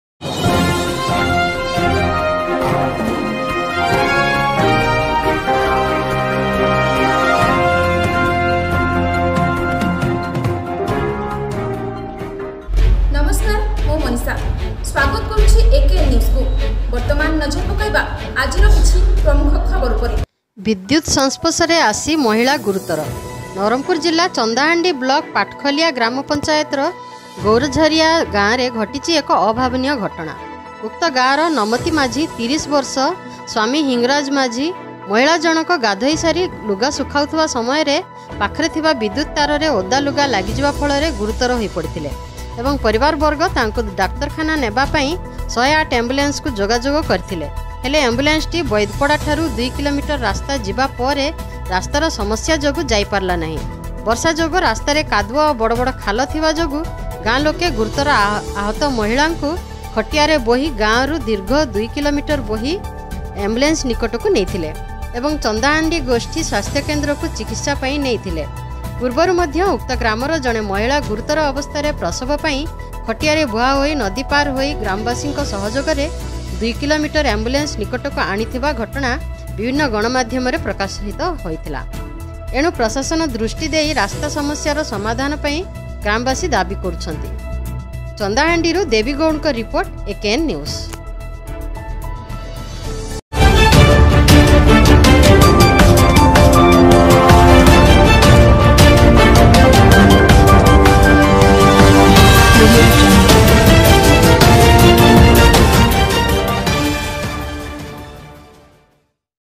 ବିଦ୍ୟୁତ ସଂସ୍ପର୍ଶ ରେ ଆସି ମହିଳା ଗୁରୁତର
ନବରଙ୍ଗପୁର ଜିଲ୍ଲା ଚନ୍ଦାହାଣ୍ଡି ବ୍ଲକ ପାଟଖଲିଆ ଗ୍ରାମ ପଂଚାୟତ ର ଗୌରଝରିଆ ଗାଁ ରେ ଘଟଛି ଏକ ଅଭାବନୀୟ ଘଟଣା ଉକ୍ତ ଗାଁର  ନମତି ମାଝୀ 30 ବର୍ଷ ସ୍ୱାମୀ ହିଙ୍ଗରାଜ ମାଝୀ ମହିଳା ଜଣକ ଗାଧୋଇ ସାରି ଲୁଗା ଶୁଖାଉ ଥିବା ସମୟରେ ପାଖରେ ଥିବା ବିଦ୍ୟୁତ ତାର ରେ ଓଦା ଲୁଗା ଲାଗିଯିବା ଫଳରେ ଗୁରୁତର ହୋଇ ପଡ଼ିଥିଲେ ଏବଂ ପରିବାର ବର୍ଗ ତାଙ୍କୁ ଡାକ୍ତରଖାନା ନେବା ପାଇଁ ଶହେ ଆଠ ଆମ୍ବୁଲାନ୍ସ କୁ ଯୋଗାଯୋଗ କରି ଥିଲେ ହେଲେ ଆମ୍ବୁଲାନ୍ସ ଟି ବୈଦପଡା ଠାରୁ ଦୁଇକିଲୋମିଟର  ରାସ୍ତା ଯିବାପରେ ରାସ୍ତା ର ସମସ୍ୟା ଯୋଗୁଁ ଯାଇପାରିଲା ନାହିଁ ବର୍ଷା ଯୋଗୁଁ ରାସ୍ତା ରେ କାଦୁଅ ଏବଂ ବଡ଼ ବଡ଼ ଖାଲ ଥିବାଯୋଗୁ ଗାଁ ଲୋକେ ଗୁରୁତର ଆହତ ମହିଳା ଙ୍କୁ ଖଟିଆ ରେ ବୋହି ଗାଁ ଠାରୁ ଦୀର୍ଘ ଦୁଇ କିଲୋମିଟର ବୋହି ଆମ୍ବୁଲାନ୍ସ ନିକଟକୁ ନେଇ ଥିଲେ ଏବଂ ଚନ୍ଦାହାଣ୍ଡି ଗୋଷ୍ଠୀ ସ୍ୱାସ୍ଥ୍ୟ କେନ୍ଦ୍ର କୁ ଚିକିତ୍ସା ପାଇଁ ନେଇ ଥିଲେ ପୂର୍ବରୁ ମଧ୍ୟ ଉକ୍ତ ଗ୍ରାମର ଜଣେ ମହିଳା ଗୁରୁତର ଅବସ୍ଥା ରେ ପ୍ରସବ ପାଇଁ ଖଟିଆ ରେ ବୁହାହୋଇ ନଦୀପାର ହୋଇ ଗ୍ରାମବାସୀ ଙ୍କ ସହଯୋଗ ରେ ଦୁଇକିଲୋମିଟର ଆମ୍ବୁଲାନ୍ସ ନିକଟକୁ ଆଣି ଥିବା ଘଟଣା ବିଭିନ୍ନ ଗଣମାଧ୍ୟମ ରେ ପ୍ରକାଶିତ ହୋଇଥିଲା ଏଣୁ ପ୍ରଶାସନ ଦୃଷ୍ଟି ଦେଇ ରାସ୍ତା ସମସ୍ୟା ର ସମାଧାନ ପାଇଁ ଗ୍ରାମ ବାସି ଦାବୀ କରିଛନ୍ତି!